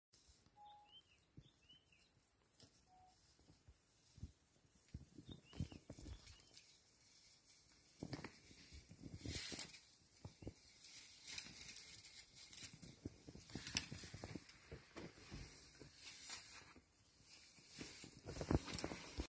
Elephant Tusker walks past research vehicle.
Our Big Trees Project Manager, Robin Cook, enjoyed an incredible sighting of Ezulwini while out on a Vulture tree survey.

Ezulwini’s name means ‘place of heaven’ , a feeling one might have when in the presence of this tusker.